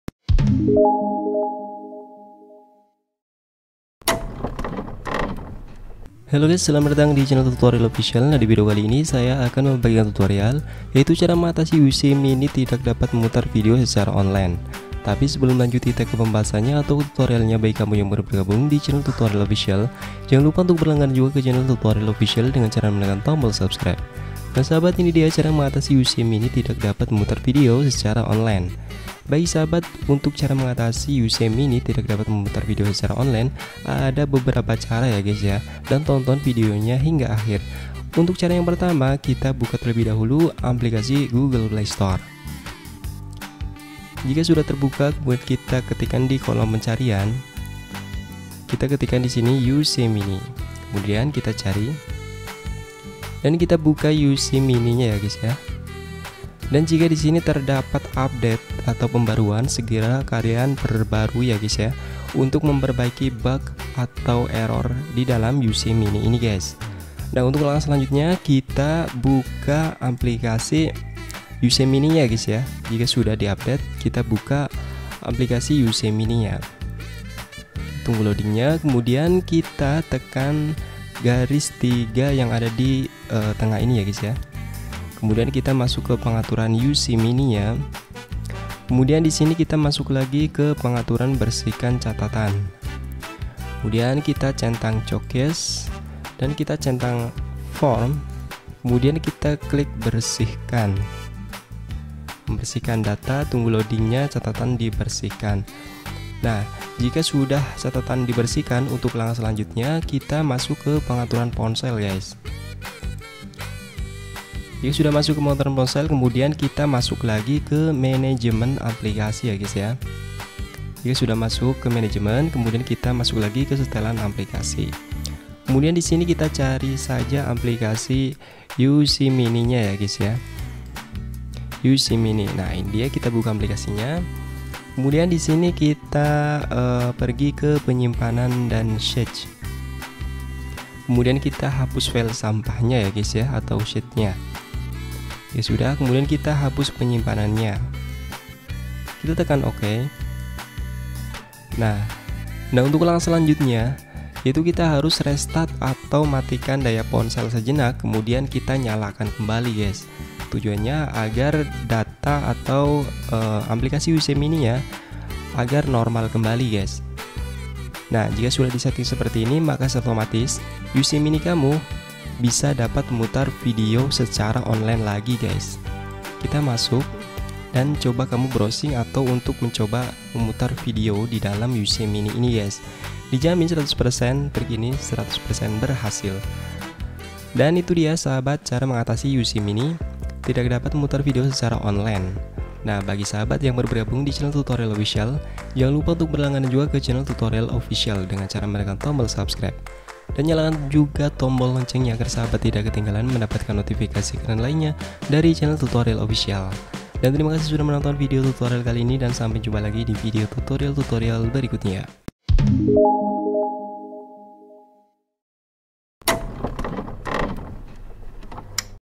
Cara mengatasi UC MINI tidak bisa memutar video online | Tutorial Official
Cara mengatasi UC MINI Tidak dapat memutar video Online perlu diketahui, Ada beberapa cara sebagai solusi untuk Cara mengatasi UC MINI Tidak dapat memutar video online atau uc miini yang error, nah berikut saya telah merangkum tutorial Cara mengatasi UC MINI tidak bisa memutar video online | Tutorial Official

agar tidak salah paham jangan skip videonya.
selamat mencoba dan semoga berhasil.